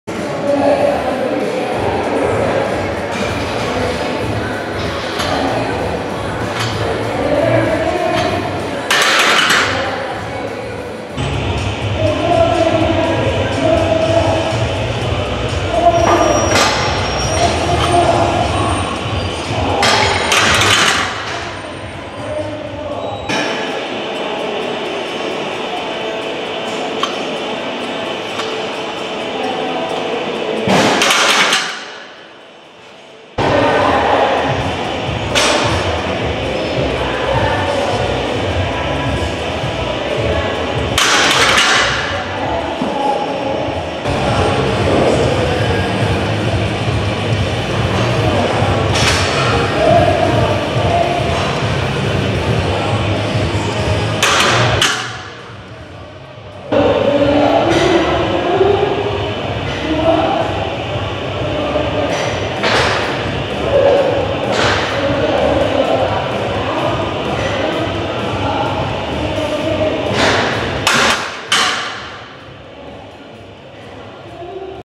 Speed Bench 85 @ 4 x 3, to 115 x 2
Doubles at RPE of 7-8, work to double @ 9 RPE. Feeling pretty good here.

85 @ 4 x 3, 105 x 2, 115 x 2